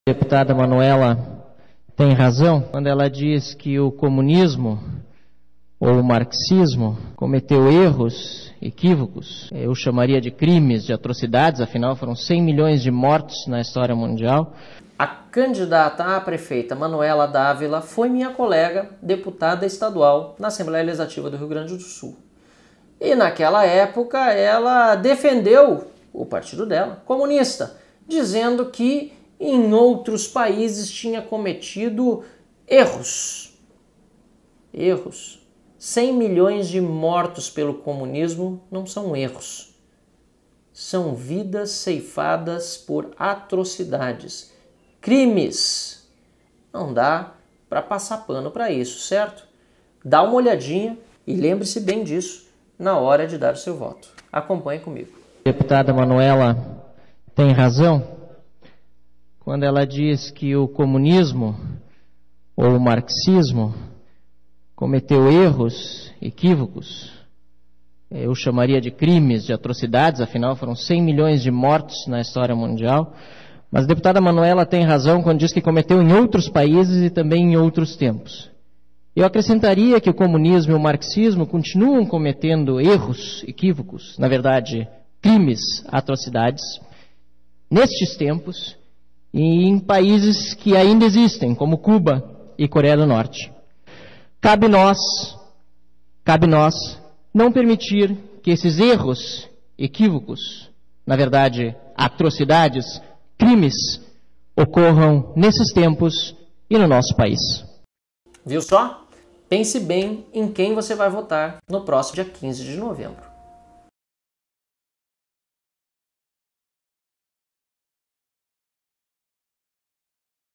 QUEM É MANUELA D'ÁVILA? | EPISÓDIO#05 MANUELA MINIMIZA ATROCIDADES DO COMUNISMO NO MUNDO
SÉRIE: QUEM É MANUELA D'ÁVILA? - EPISÓDIO#05 MANUELA MINIMIZA ATROCIDADES DO COMUNISMO NO MUNDO

A candidata comunista Manuela D'Ávila atribuiu, quando deputada, como meros "erros" as atrocidades cometidas pelos regimes comunistas no mundo ao longo dos últimos 100 anos. Há relatos e documentos fidedignos que estimam mais de 100 milhões de mortes causadas pelos tiranos comunistas no mundo. Desde a Revolução dos Bolcheviques em 1917 barbáries promovidas por regimes comunistas se sucederam pelo mundo. Stálin ceifou milhões de ucranianos por fome, mediante confisco de comida. Isso sem falar nos famosos expurgos e tiros na nuca na URSS. Pol Pot trucidou o povo do Camboja. A grande fome na China do comunista Mao causou, estima-se, 45 milhões de mortes.

Na primeira metade do século passado, o ídolo comunista brasileiro Luís Carlos Prestes tentou transformar o Brasil num Camboja continental com a Intentona Comunista e chegou a cumprir pena por ser mandante da execução de uma jovem correligionária de 17 anos de idade, mas ganhou recentemente um memorial em Porto Alegre - fato que foi aplaudido pelo PCdoB. Um século de atrocidades que, para Manuela, são apenas "erros".

Não, Manuela, não foram meros erros. Foram crimes, ATROCIDADES!